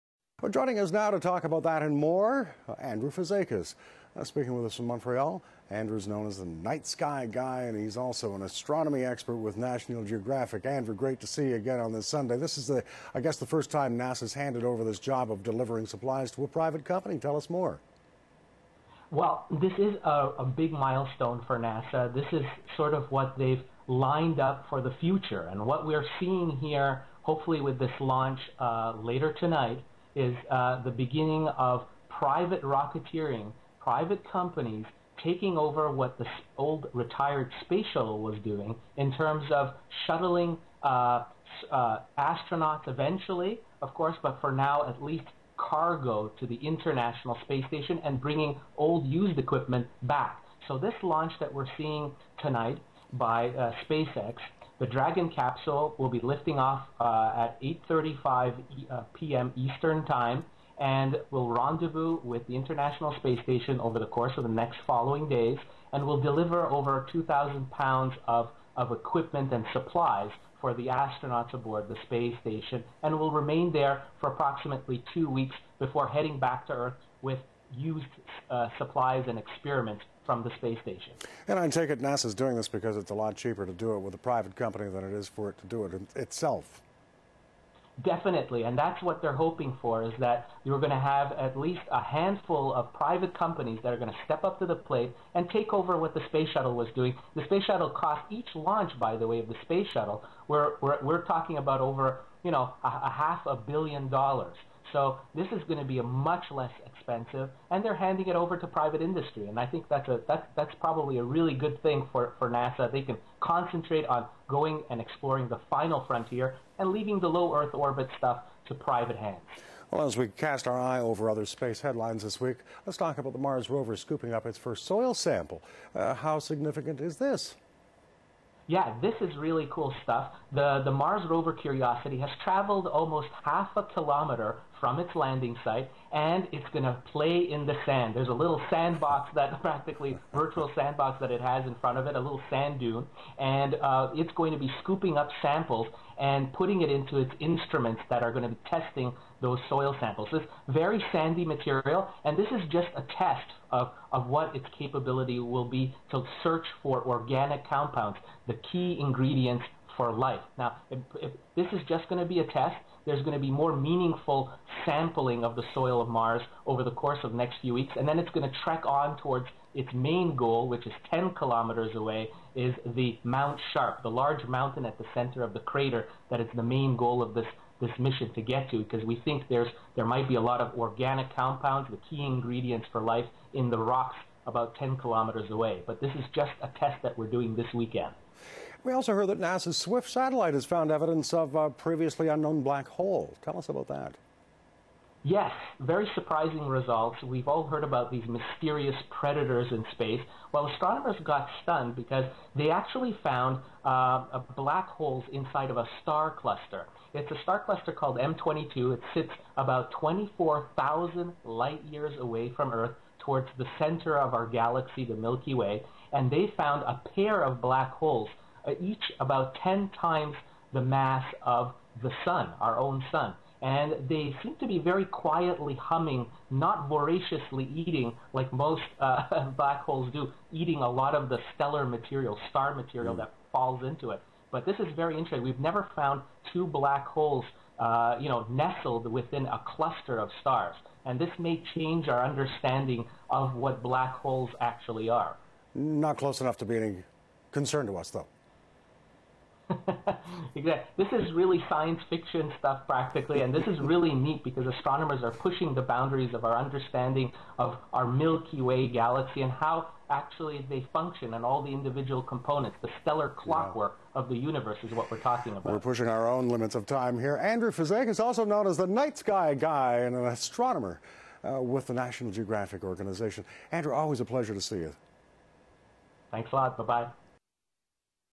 Mars rover, SpaceX launch and Black Hole News Interview Oct 7 2012
This week's space news roundup includes the first Martian soil sampling by Curiosity, first official cargo run to the ISS by SpaceX and pair of black holes found buried inside a giant star cluster 22,000 light years from Earth.